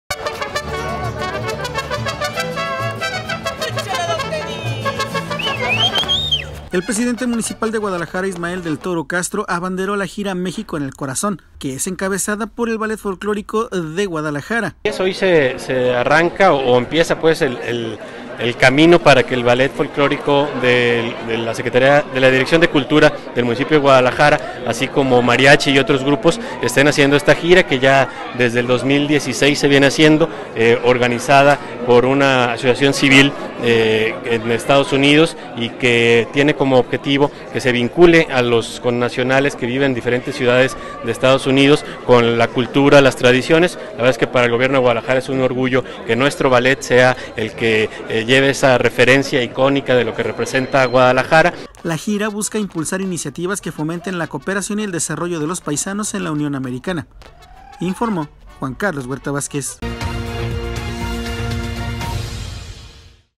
Representarán a Mexico
Alcalde de Guadalajara, Ismael Del Toro Castro, abanderó la gira “México en el Corazón”, integrada por el Ballet Folclórico de Guadalajara